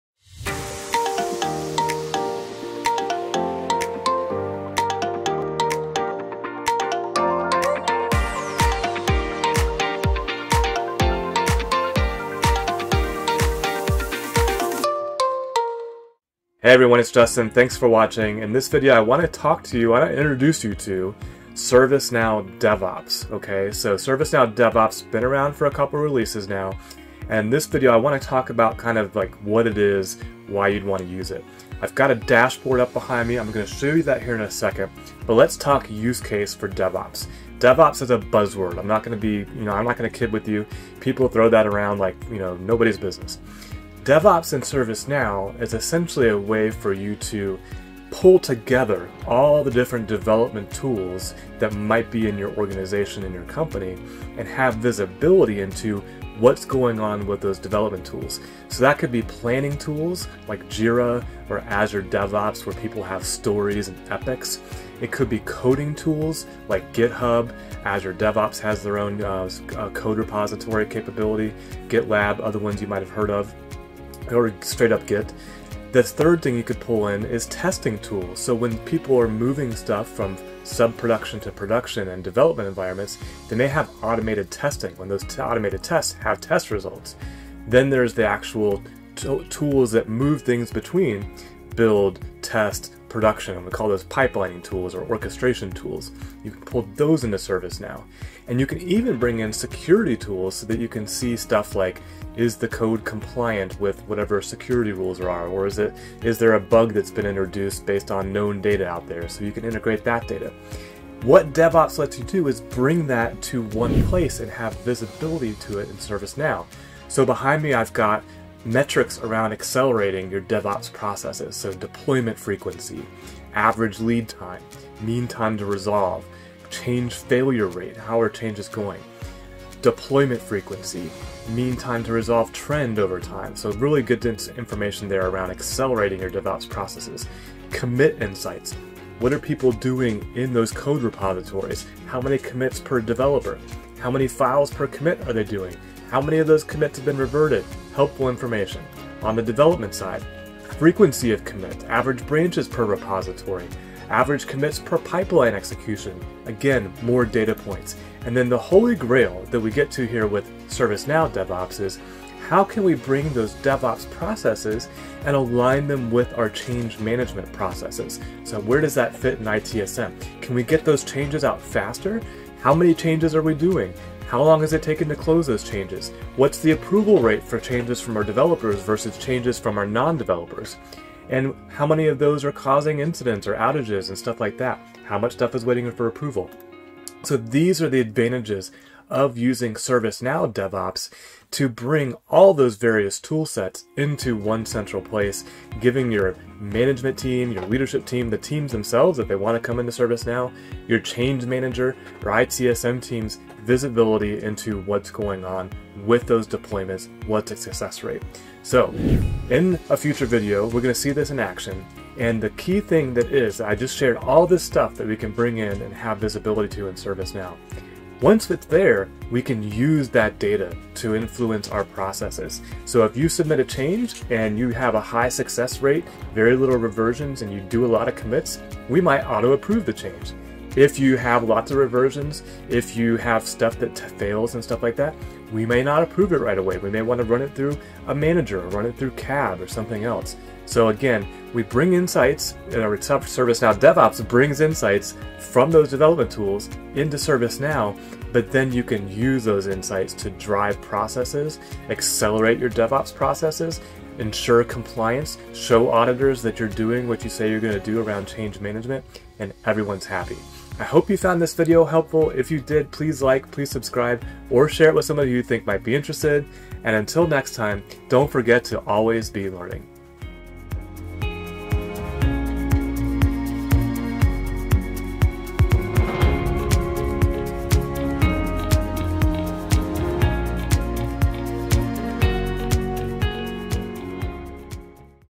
ServiceNow DevOps Introduction
You've probably heard the word DevOps but do you want to know what ServiceNow DevOps is and how to use it? In this introduction I review the DevOps Insights dashboard and discuss why you might be interested in DevOps and how it can help your development processes.